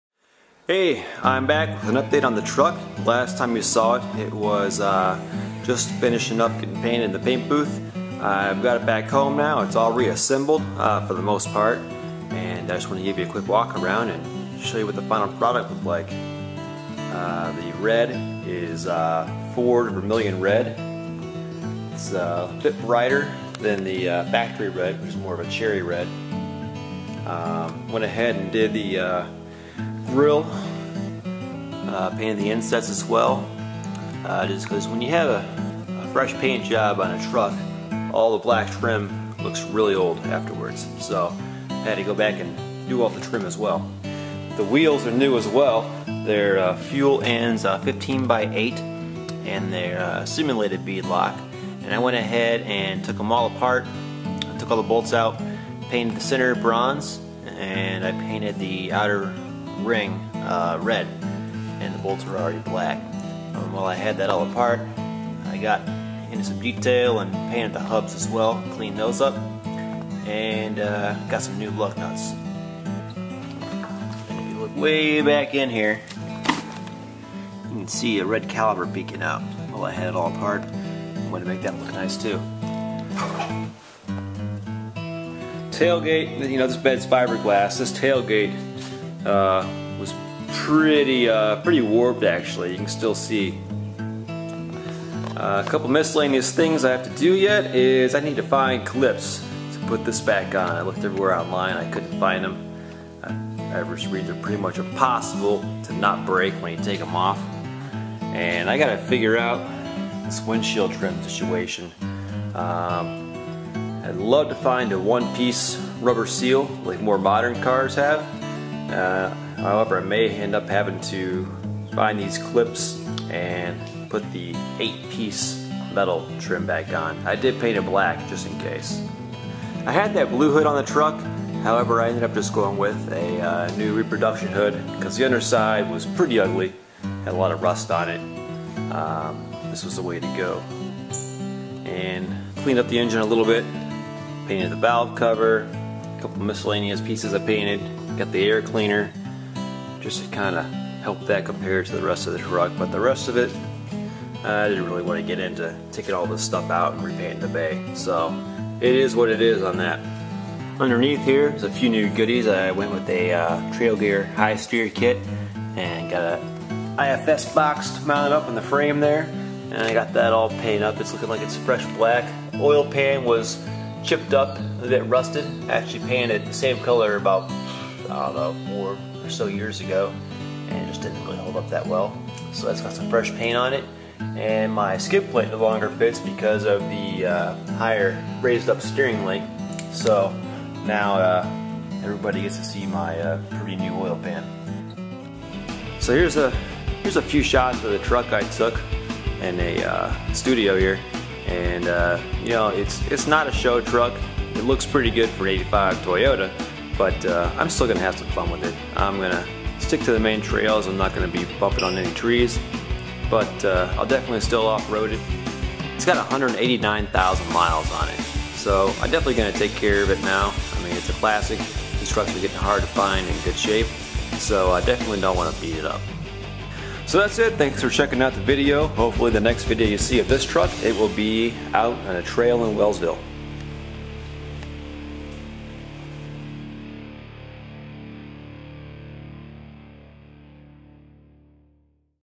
1985 Toyota Pickup Truck 4x4 - Fresh Red Paint Job!
My '85 is back from the paint booth! Here is a walk around, followed by some nice photography of the truck while it is looking its best ever.

Modifications:
15x8" Fuel Anza wheels
33x10.5" BFG Terrain T/A KO2 Tires
35x12.5" Dick Cepek Mud Country tires have been discontinued, replaced by Extreme Country
Pro Comp 4" Lift
2" Body Lift
Trail Gear Hi Steer Kit
IFS Steering Box Frame Mount plates
LED headlights
The harness for the LED headlights
Bed Armor Sandstorm Aerosol (sprayed on top of roll-on kit)
LCE Exhaust Studs
K&N Air Filter (for 22R)
Hi-Lift Jack
1994 v6 4runner master cylinder
1988 IFS rear axle (3" wider)
SKY IFS front widening kit
Front Range offroad twin-stick
1995 4runner v6 calipers
Land Cruiser FJ60 vented rotors
IFS hubs
5.29 Yukon Gears
Front axle - Aussie locker
Rear axle - ARB air locker